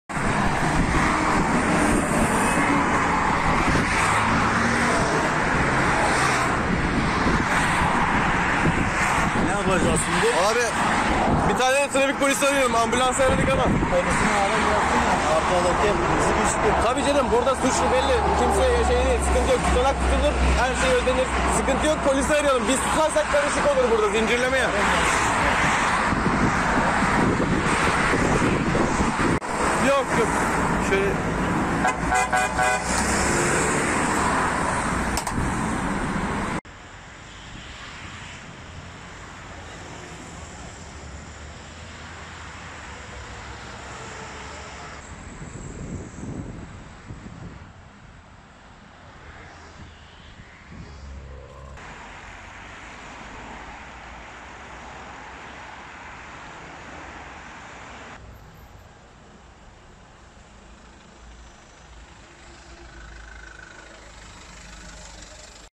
D-100’de 5 araç birbirine girdi: 1 yaralı
Gebze'de D100 Karayolu'nda 5 aracın çarpışması neticesinde meydana gelen kazada 1 kişi yaralandı.